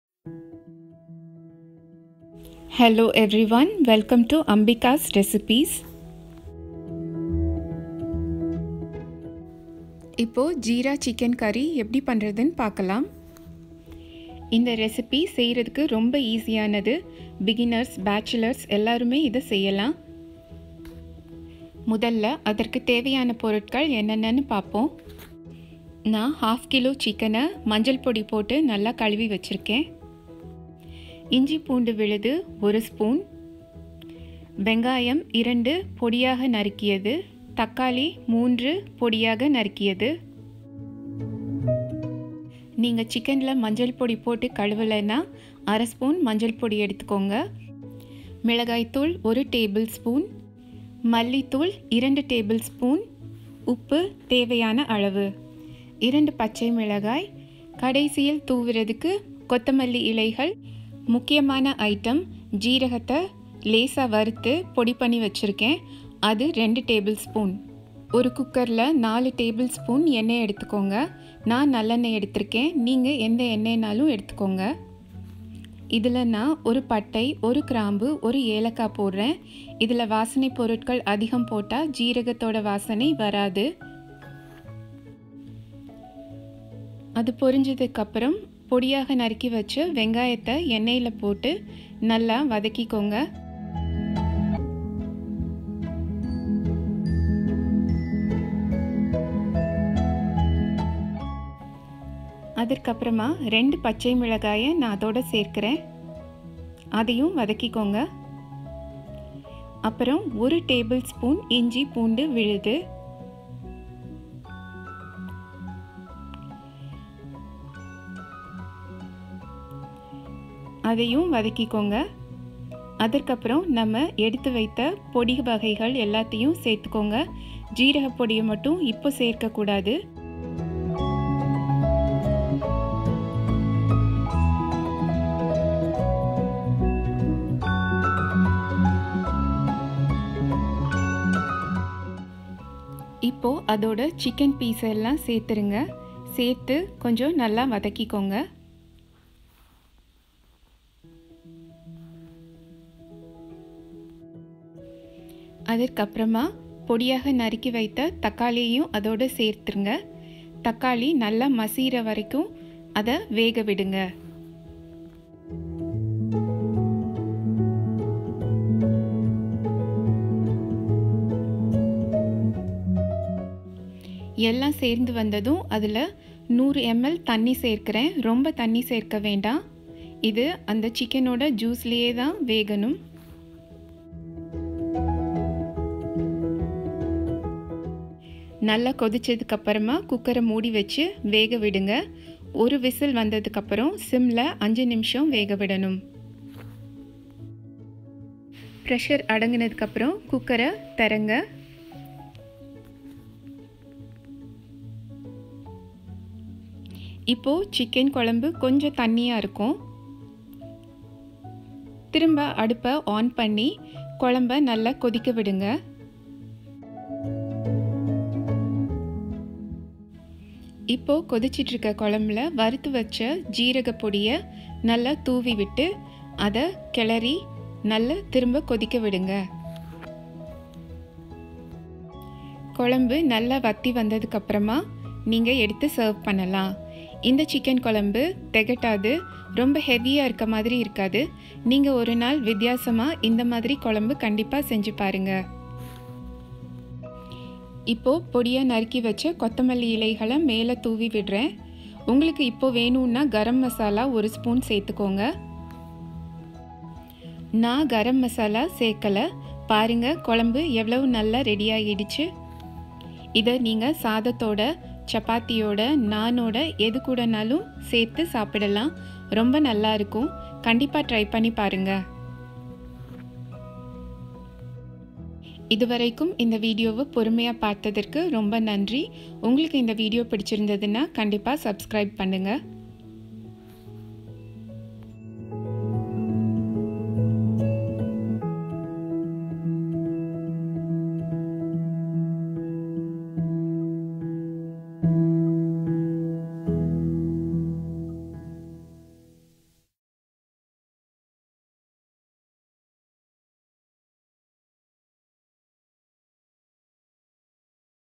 இப்படி சுவையாக சிக்கன் செய்து பாருங்கள் | Jeera Chicken Curry Recipe in Tamil
Make this easy tasty jeera chicken at home. Ingredients in Video. Subtitles in English.
Credits : iMovie